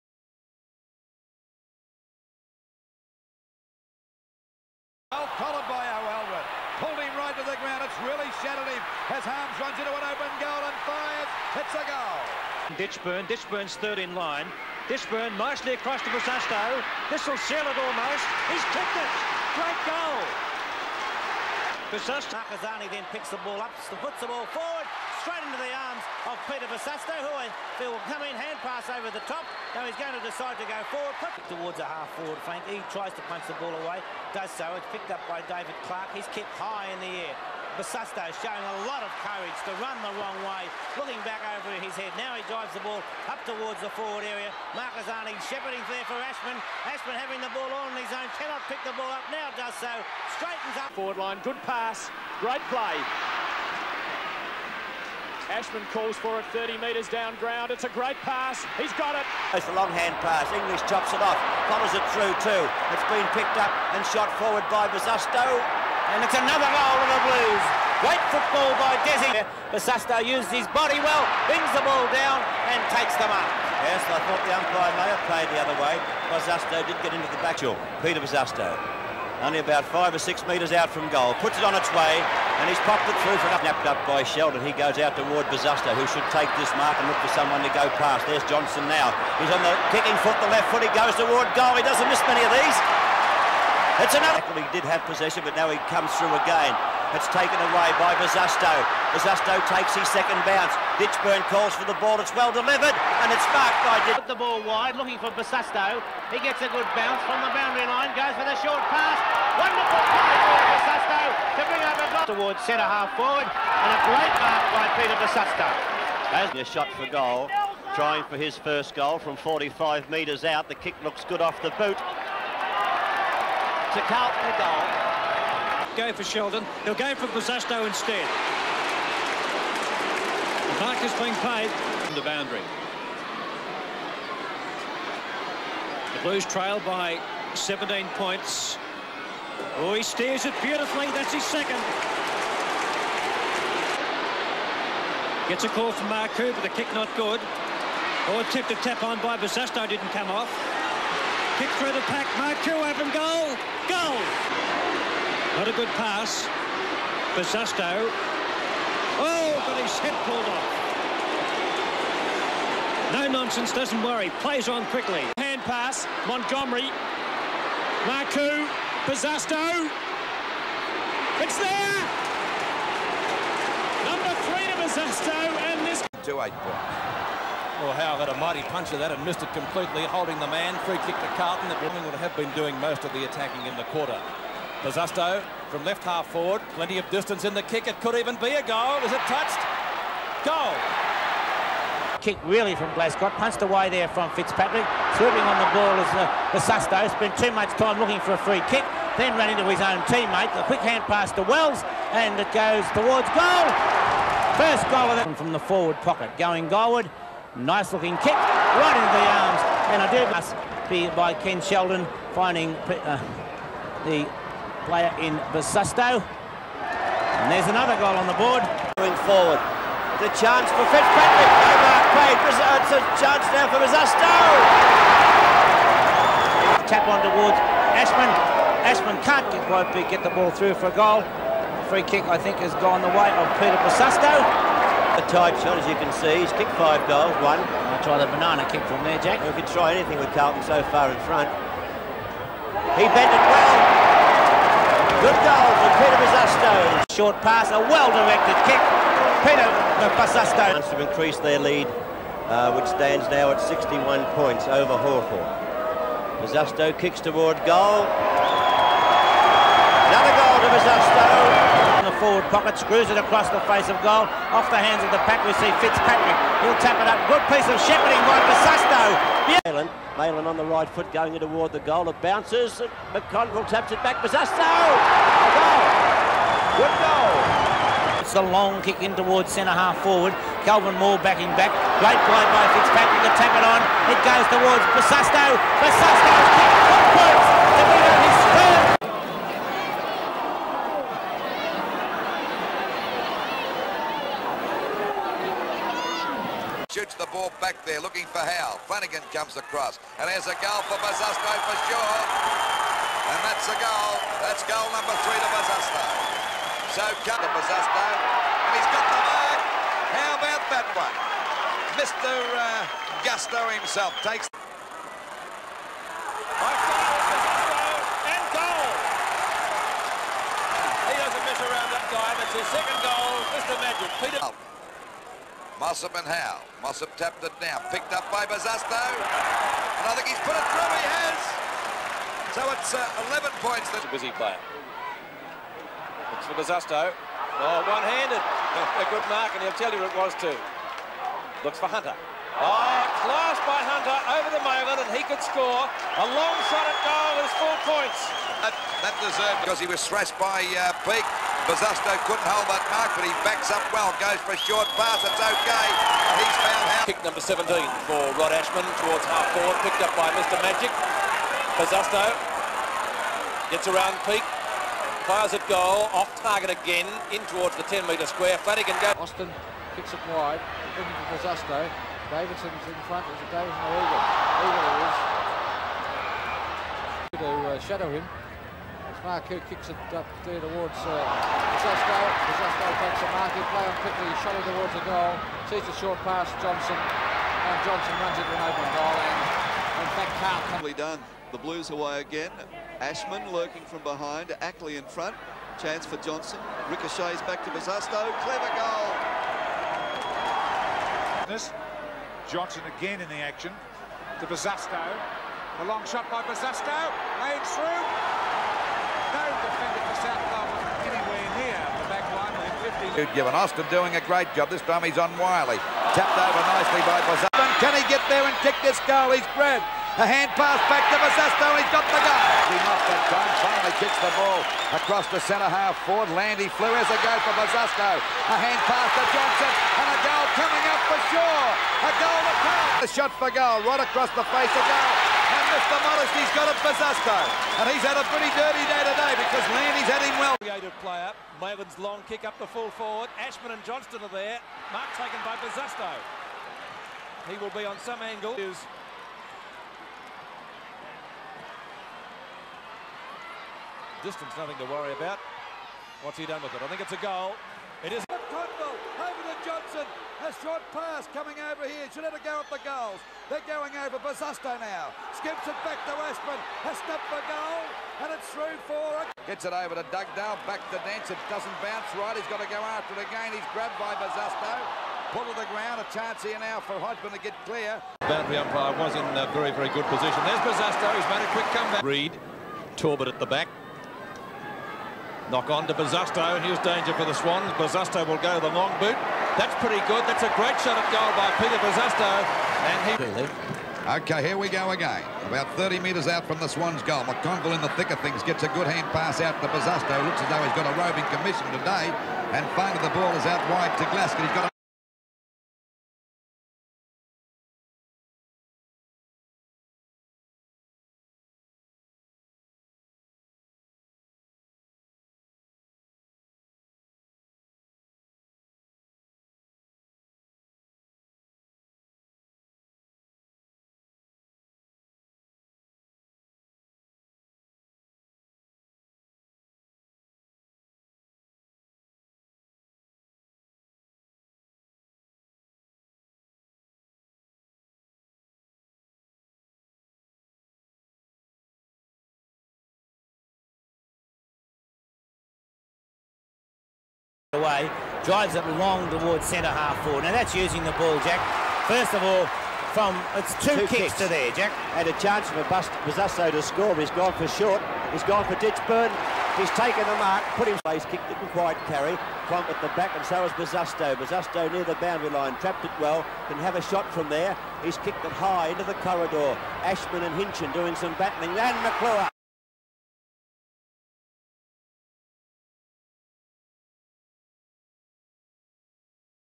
Peter Bosustow 1982 - Carlton Football Club Past Player
Vision from Peter Bosustow's 1982 VFL season for the Carlton Football Club.